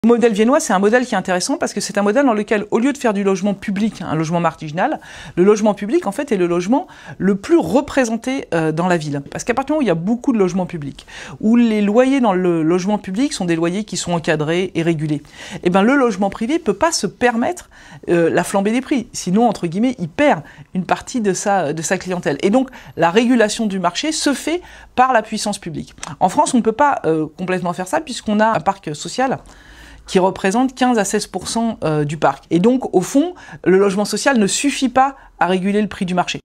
BestOf2022 : 🇦🇹 Vienne, ville idéale pour se loger ? - 27
À Vienne, c’est le logement social qui dicte la loi du marché. Et le privé suit le pas 🏠 Un modèle à suivre selon Marianne Louis, Directrice générale de l’@unionhlm